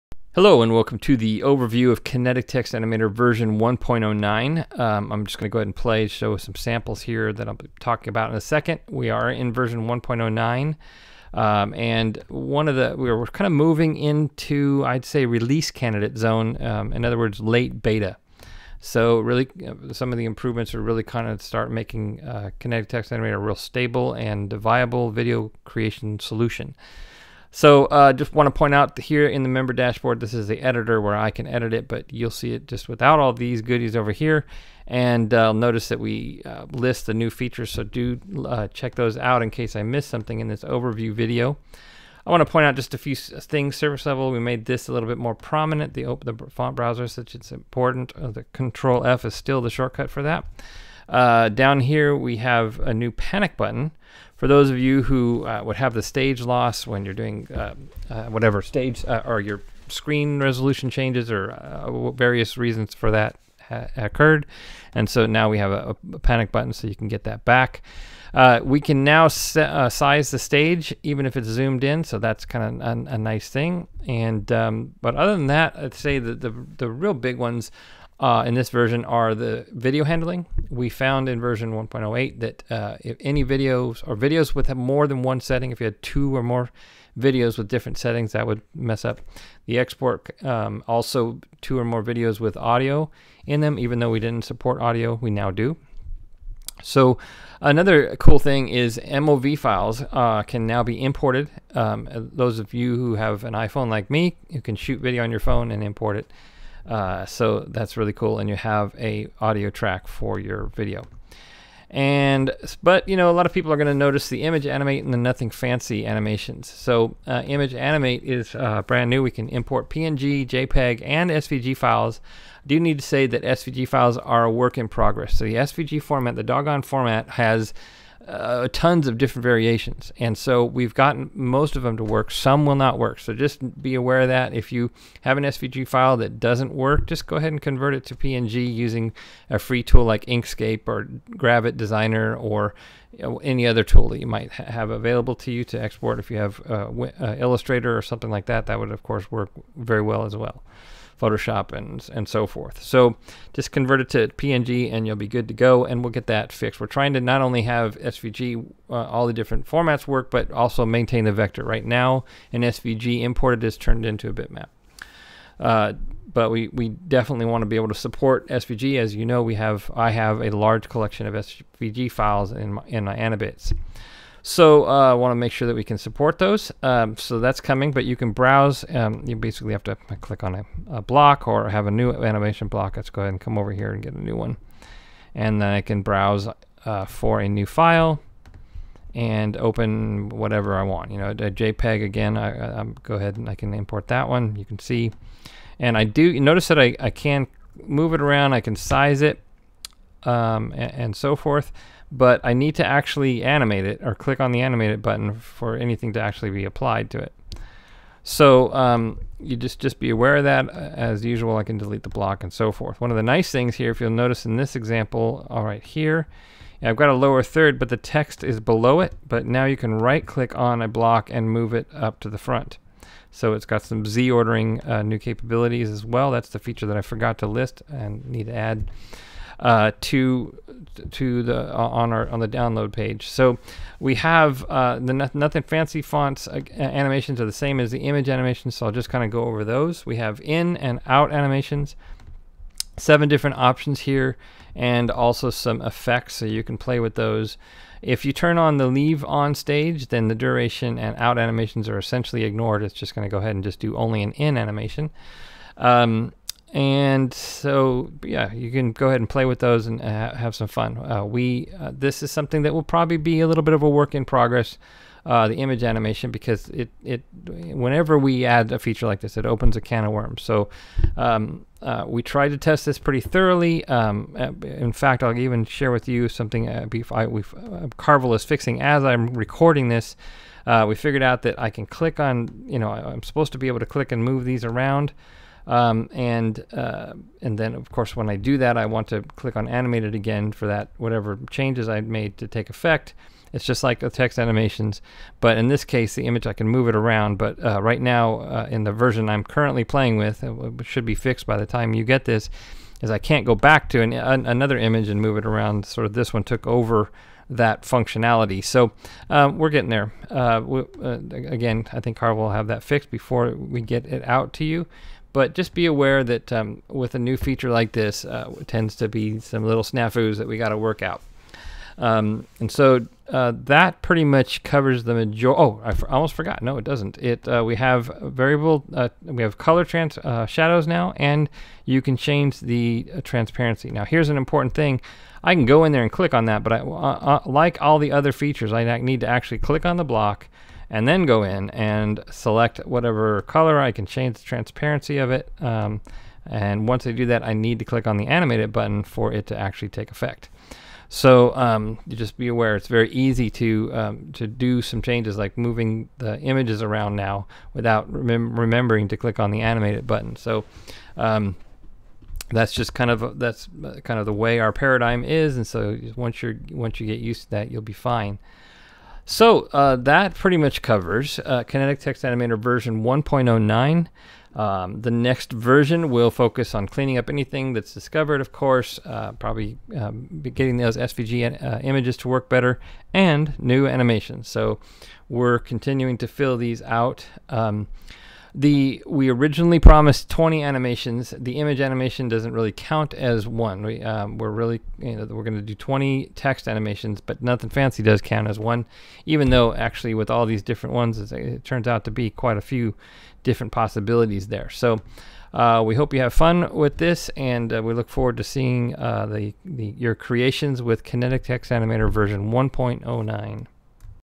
What's New In Kinetic Text Animator 1.0.9
This is an overview of what's new in Kinetic Text Animator version 1.0.9